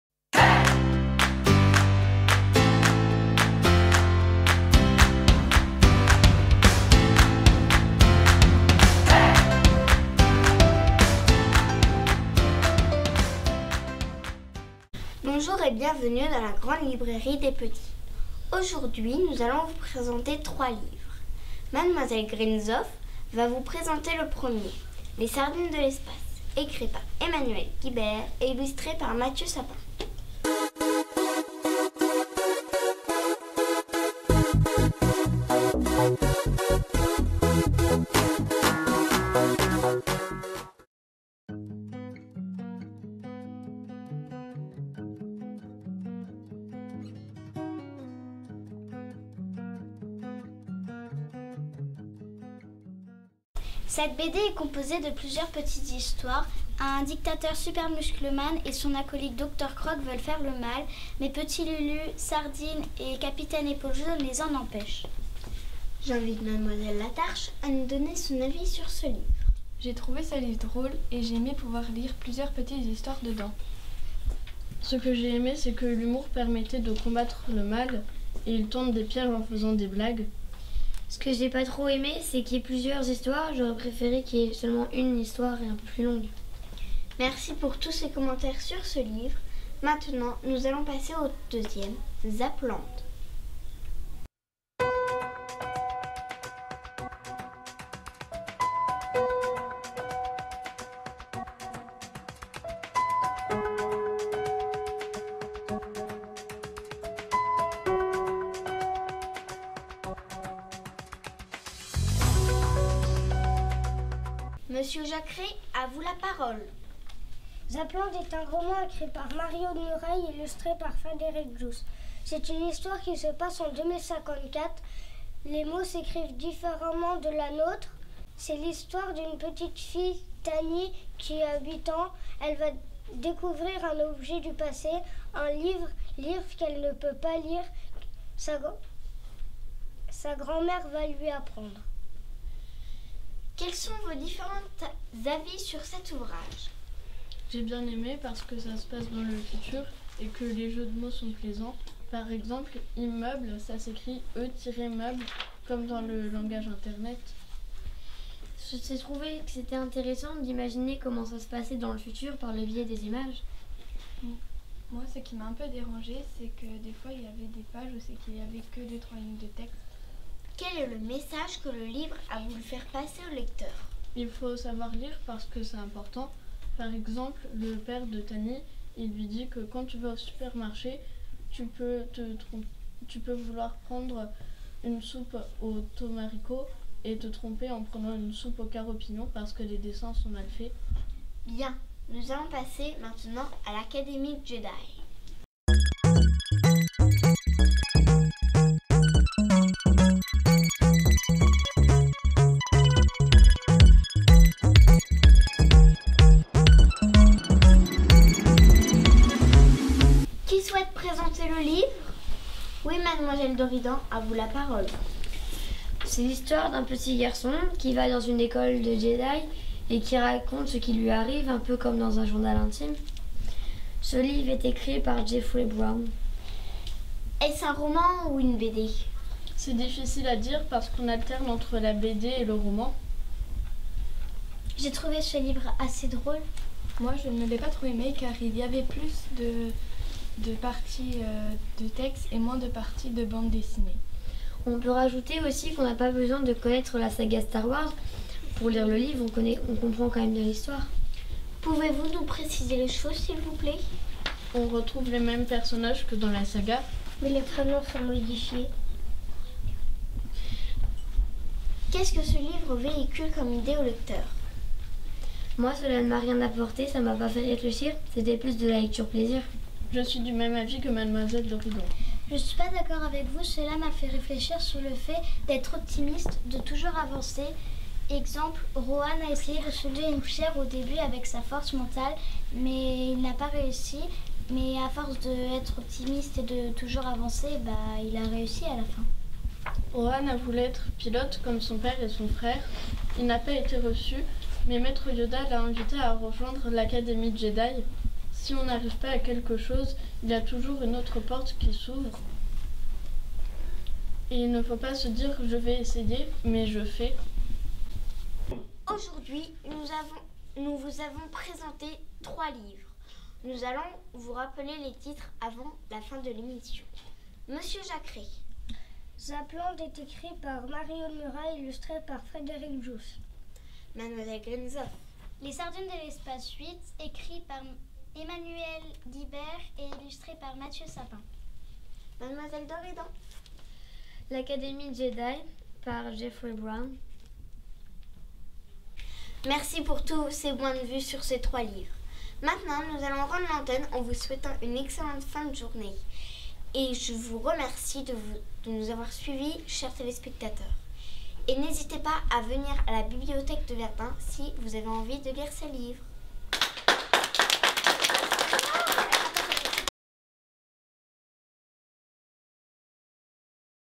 La Grande Librairie des Petits - Ecole Jules Ferry de Verdun
La Grande Librairie des Petits, ce sont des élèves de CM1 ET CM2 , tous issus d'écoles de la Meuse, qui lisent, analysent et critiquent des livres... comme des grands ! Et pour que ça fasse encore plus vrai, ils ont même écrit, enregistré et réalisé leur propre émission, comme à la télévision.

Les élèves de l'école Jules Ferry de Verdun, en classes de CM1 et CM2, présentent une sélection de livres autour de la science-fiction.

Sardine de l'Espace, vol. 8 par Emmanuel Guibert et Mathieu Sapin